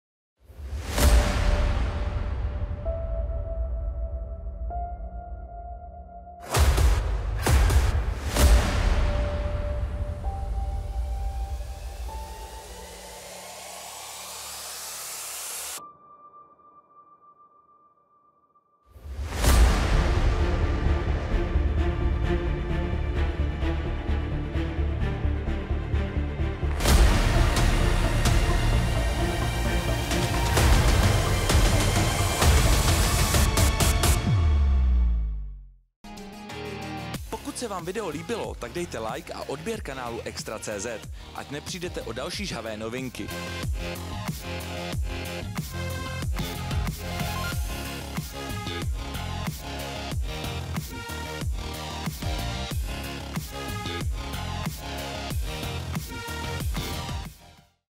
Tvrdá dřina do roztrhání! Lucka Pudilová na UFC v Praze oslní nejen krásou, ale…
Nejlepší česká MMA bojovnice Lucie Pudilová (24) je v tvrdé přípravě na svůj pátý zápas v organizaci UFC. V disciplíně zvané MMA se střetne s obávanou Liz Carmouche, a to 23. února na domácí půdě. Redakci eXtra.cz dala nahlédnout pod pokličku svého hodně náročného tréninku!